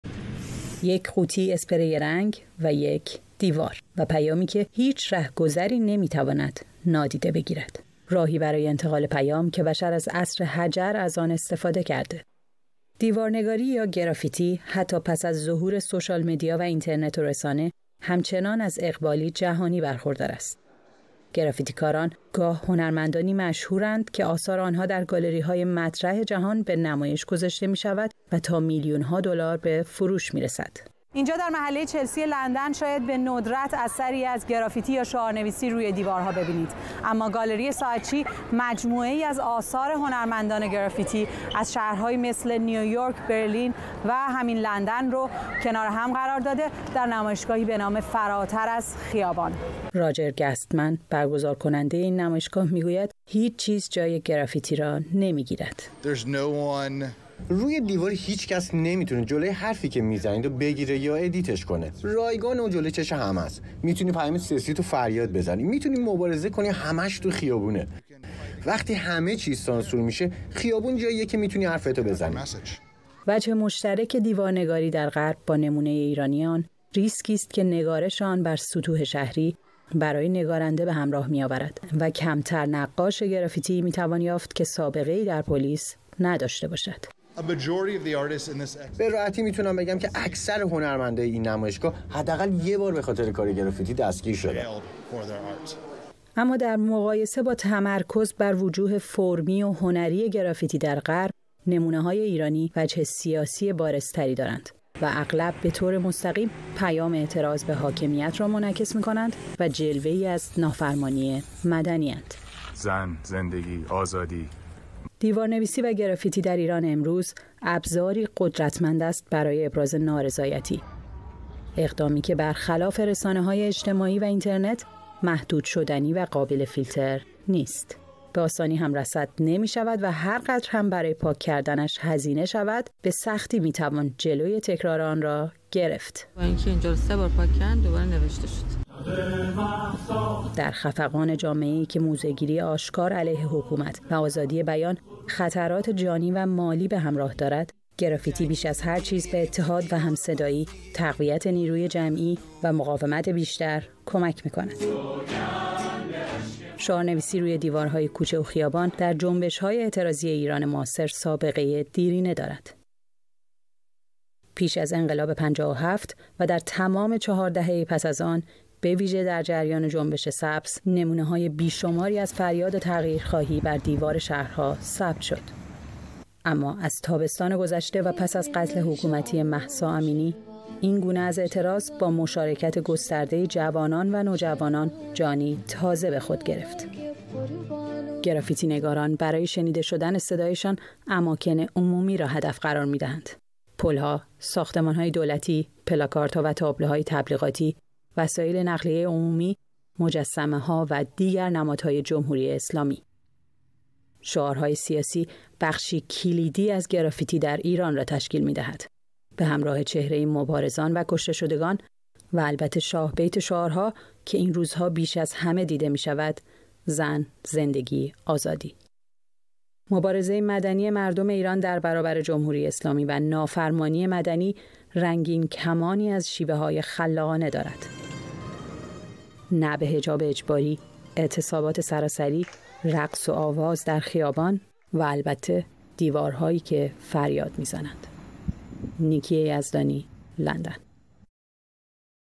نافرمانی مدنی از راه گرافیتی
شعارنویسی سیاسی بخش اعظم گرافیتی در ایران است.

گزارش نیکی یزدانی، ایران‌اینترنشنال از نمایشگاه گرافیتی به نام «فراتر از خیابان» در گالری ساعتچی، لندن